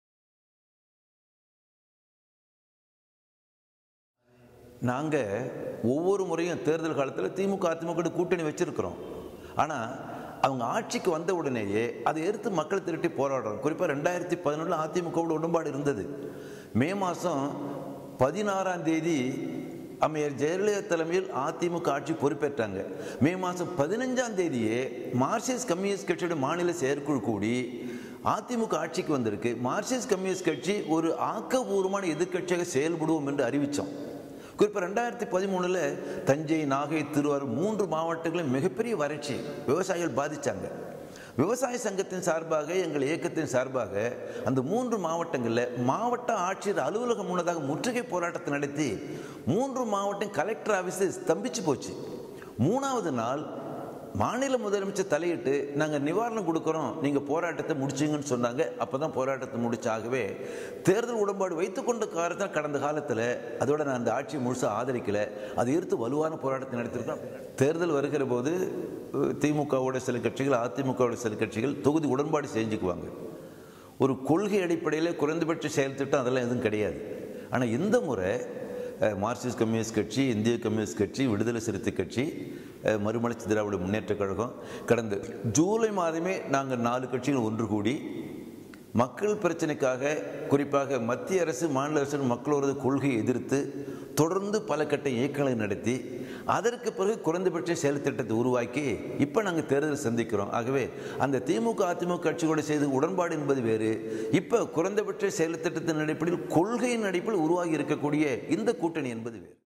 திமுக அதிமுகவோடு இருந்த அணிகளில் இருந்து ம.ந.கூ வேறுபட்டது : ஜி.ராமகிருஷ்ணன்
திமுக அதிமுகவோடு ஏற்கனவே அணியில் இருந்தது பற்றி பலரும் விமர்சிக்கின்றனர். அதற்கு தோழர் ஜி.ராமகிருஷ்ணனின்